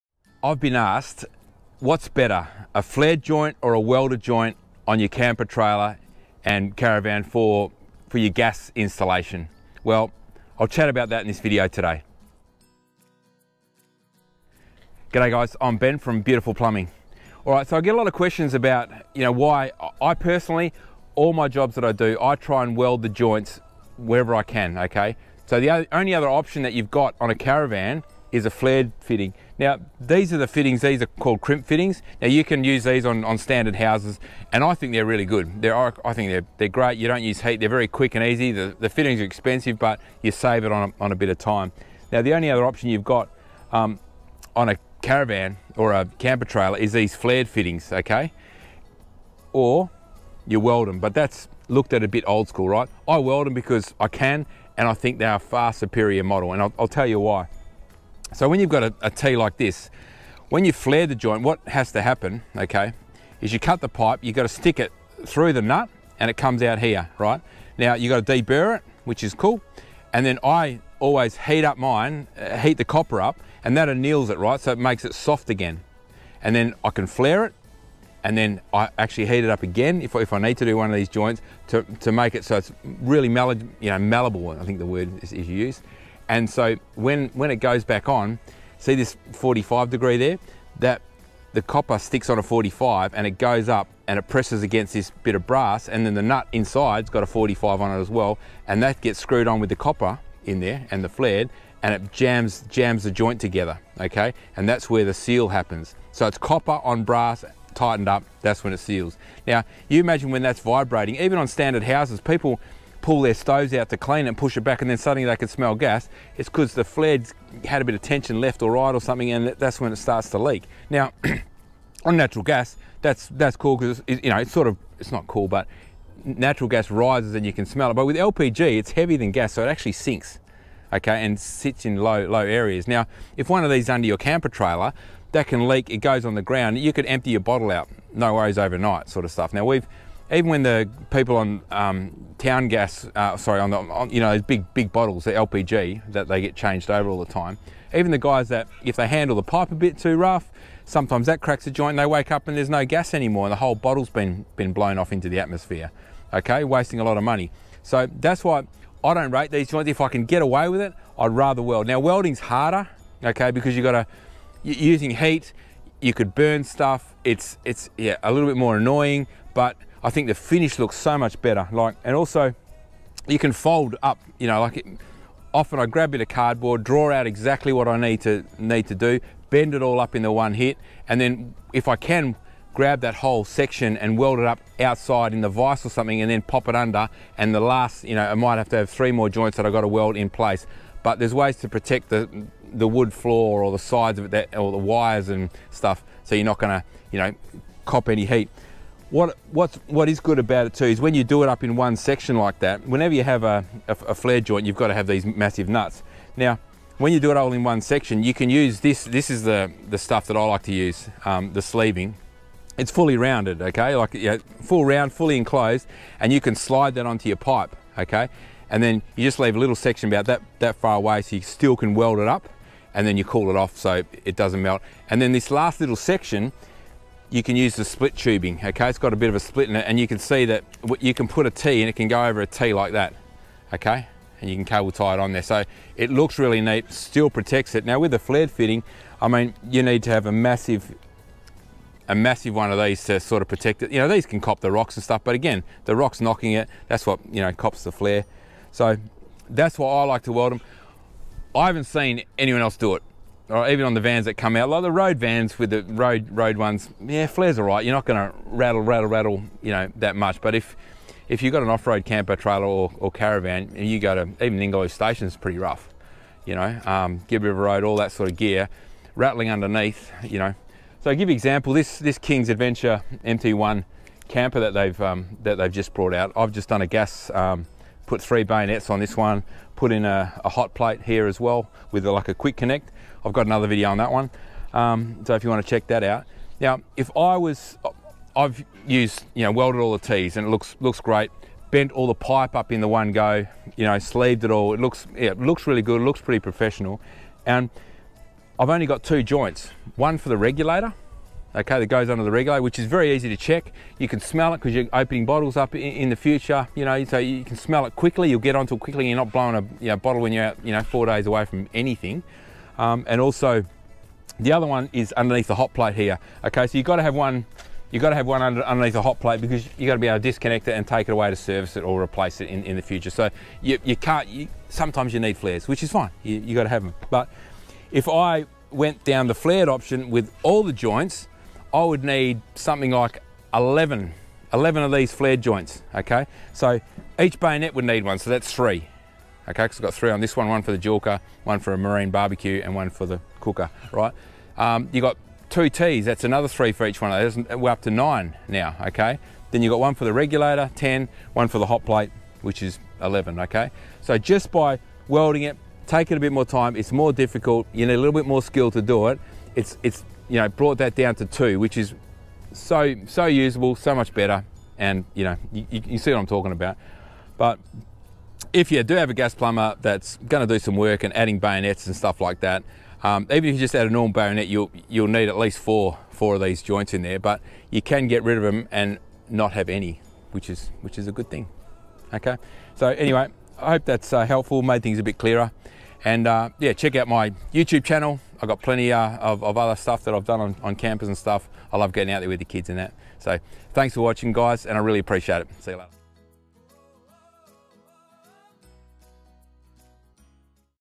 Flared Fitting vs. Welded: Which One is Better?
Which one is better, flared fitting or welded one? Check out this video to find out more!